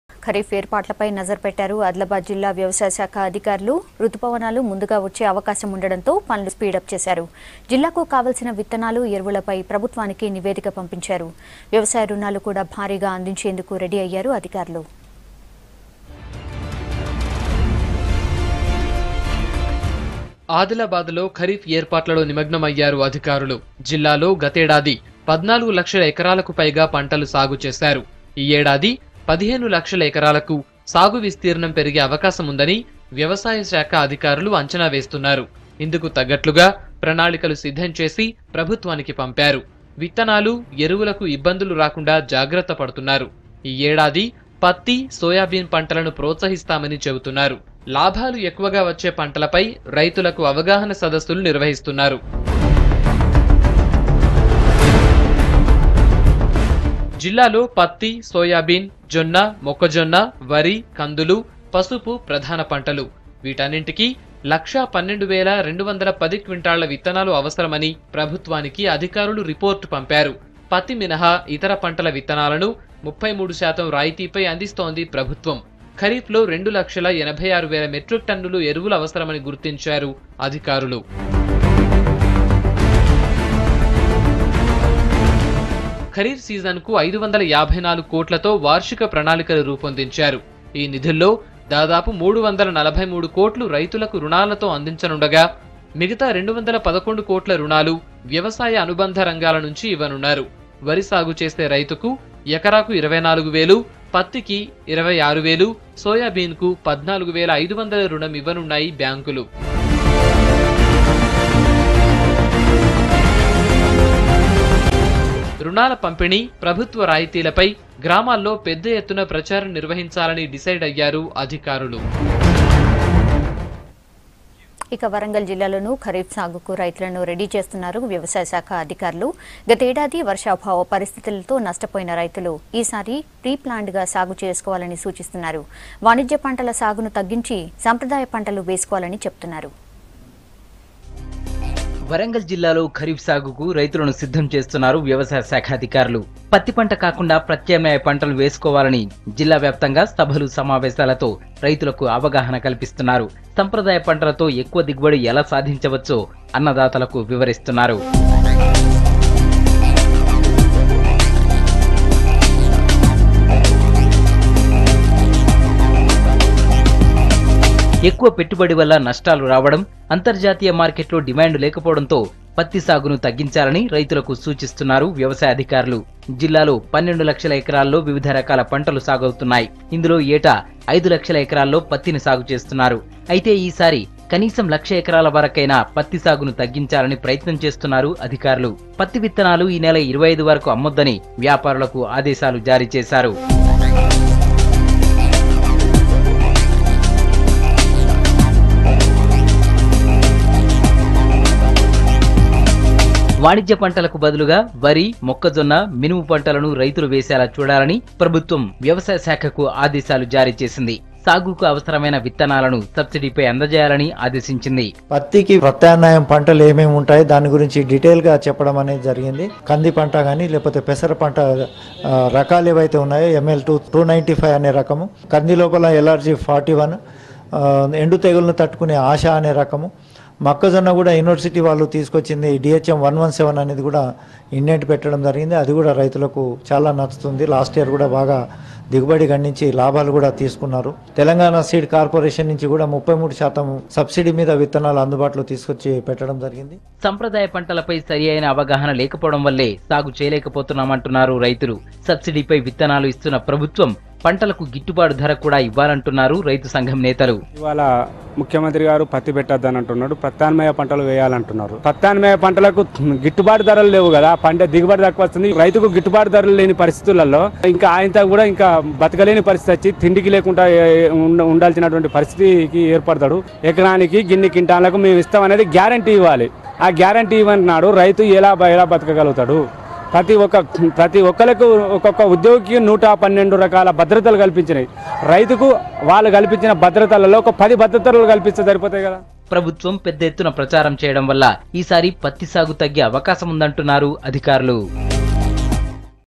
Adilabad Agriculture Dept Officials Focus On Upcoming Kharif Season | V6 News
Adilabad district agriculture department officials now concentrated on upcoming Kharif season. Officials sent a report on seed requirement and on other issues to the State agriculture ministry.